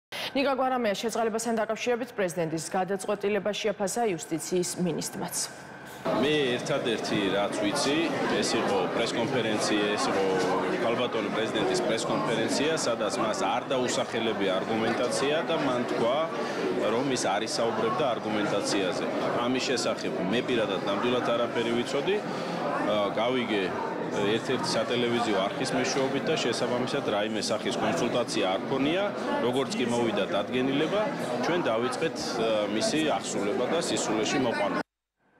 იუსტიციის მინისტრის განცხადება
ნიკა გვარამიას შეწყალებასთან დაკავშირებით პრეზიდენტის გადაწყვეტილება შეაფასა იუსტიციის მინისტრმა რატი ბრეგაძემაც.